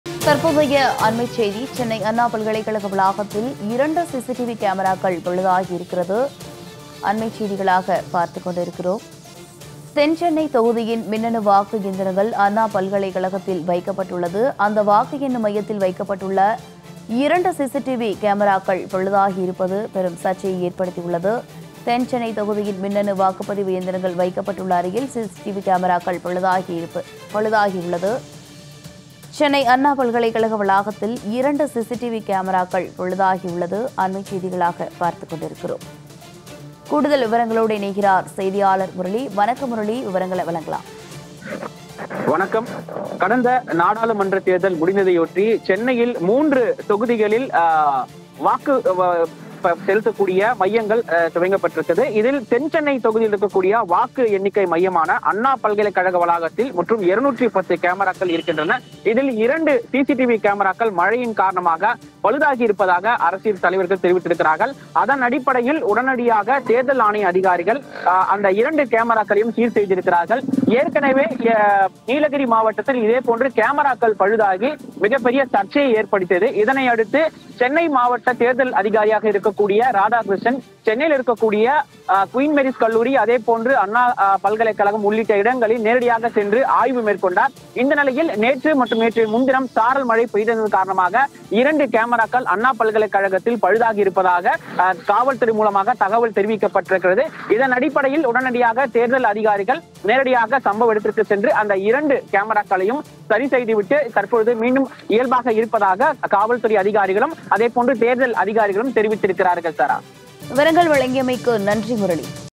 சென்னை அண்ணா பல்கலைக்கழக வளாத்தில் 2 சிசிடிவி கேமராக்கள் பழுது | vote | vvpat | cctv | Jaya Plus
சென்னை அண்ணா பல்கலைக்கழக வளாத்தில் 2 சிசிடிவி கேமராக்கள் பழுது | vote | vvpad | cctv | Jaya Plus